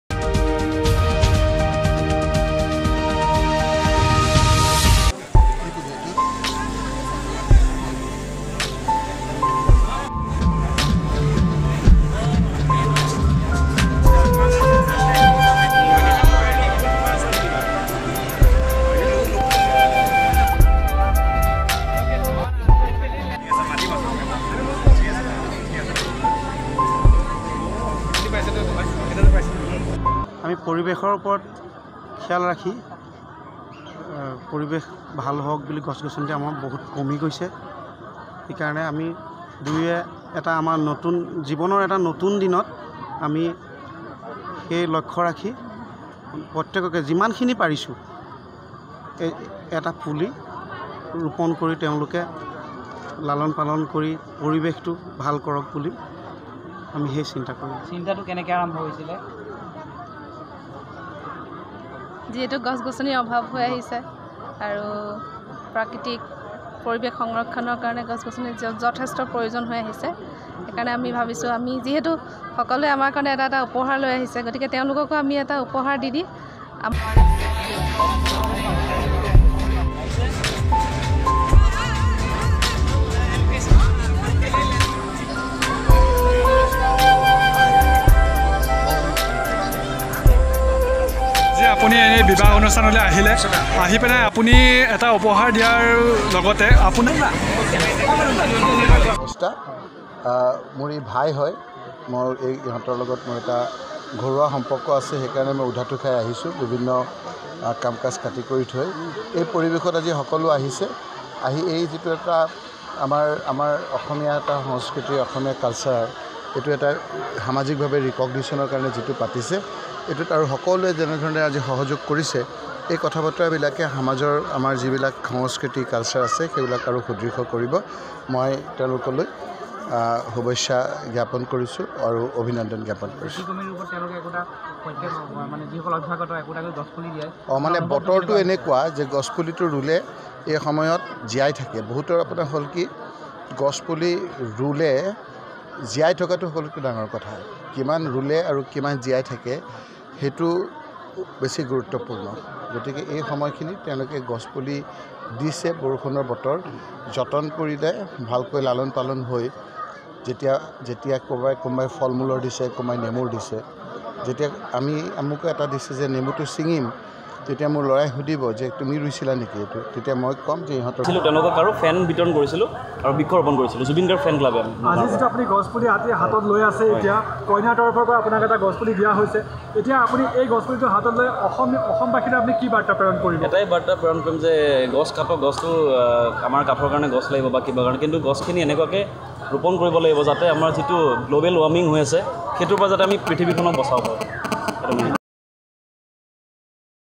A newly married couple from Babejia sets a new trend by distributed saplings to all their guest
A unique marriage reception was held at Babejia, Nagaon where newly married and environmentally conscious Couple Jitul Das and Pinky Devi Das distributed saplings of various species to all guests in attendance